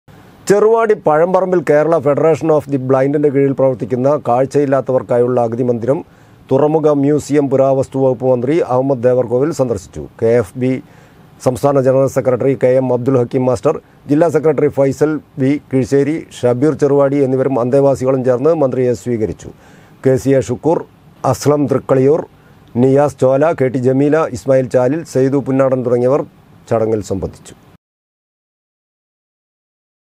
അഗതിമന്ദിരം തുറമുഖ മ്യൂസിയം പുരാവസ്തു വകുപ്പ് മന്ത്രി അഹമ്മദ് ദേവർകോവിൽ സന്ദർശിച്ചു
ചെറുവാടി, പഴംപറമ്പിൽ കേരള ഫെഡറേഷൻ ഓഫ് ദി ബ്ലൈന്റിന്റെ കീഴിൽ പ്രവർത്തിക്കുന്ന കാഴ്ച ഇല്ലാത്തവർക്കായുള്ള അഗതിമന്ദിരം തുറമുഖ മ്യൂസിയം പുരാവസ്തു വകുപ്പ് മന്ത്രി  അഹമ്മദ് ദേവർകോവിൽ സന്ദർശിച്ചു